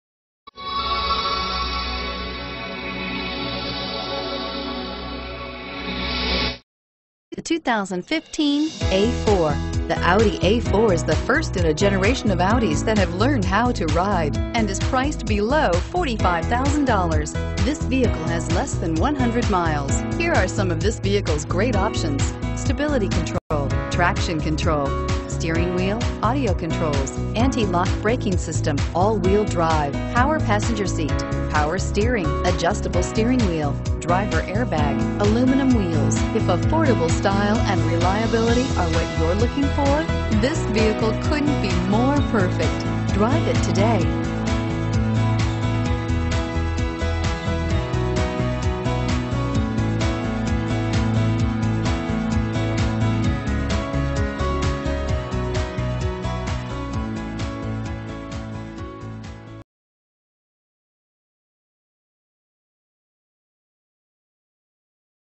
2015 Audi A4 Metairie LA N026070
2015 Audi A4 Premium Plus

You'll love this 2015 Audi A4. This is a car you'll want to take home. With 9 miles, it features Automatic transmission and an exterior color of Moonlight Blue Metallic. Call and get in touch with New Orleans Audi directly, and be the first to open the car door today!